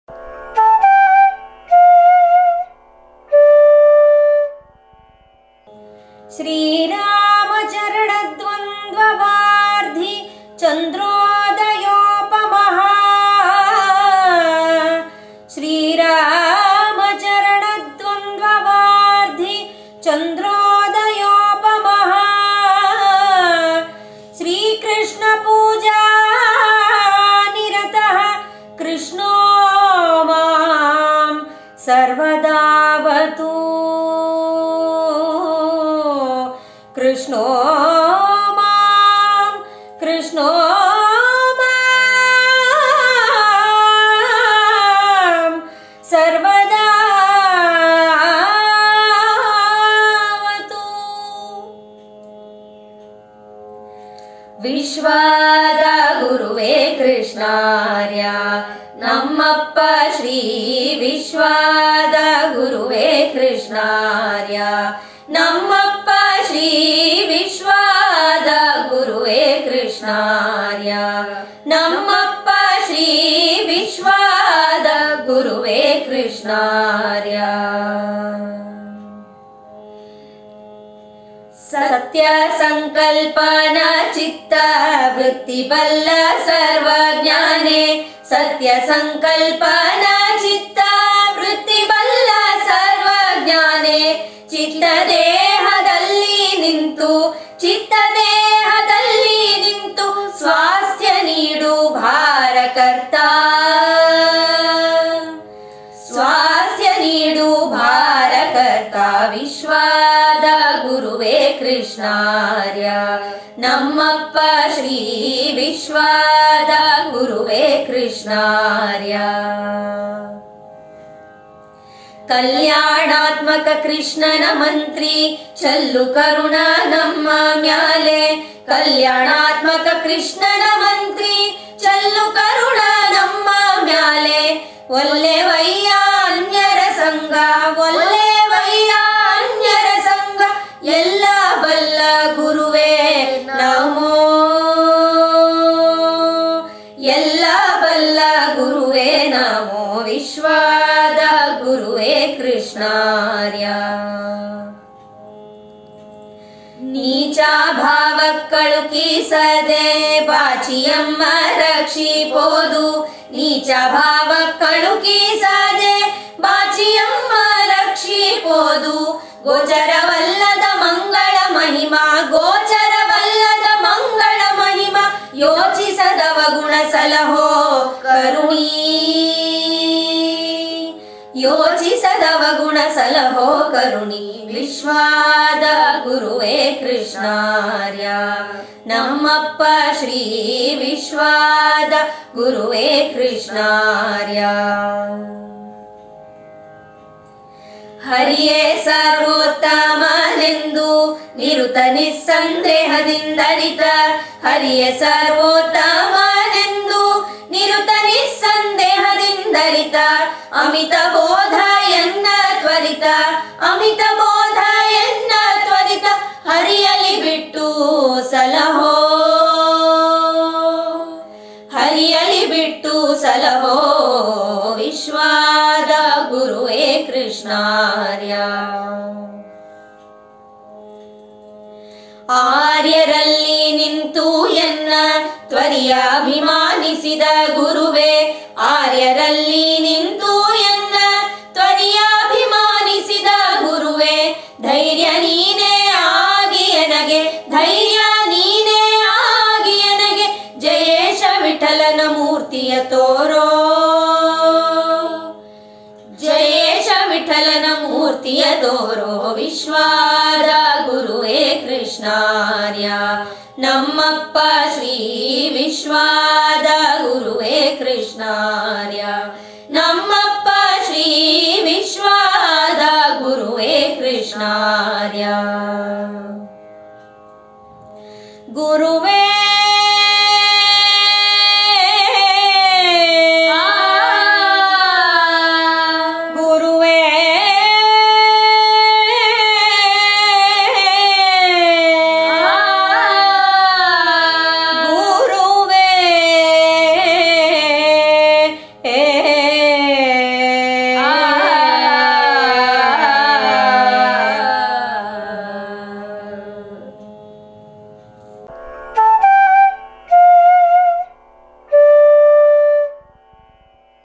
Chaturmasya vishesha sangeeta Seva-23 by Smt|| Sowmya, Kum|| Samiksha & Kum|| Sridhanya
on the special occasion of Ibramhpur Appavara Aradhane ,this song .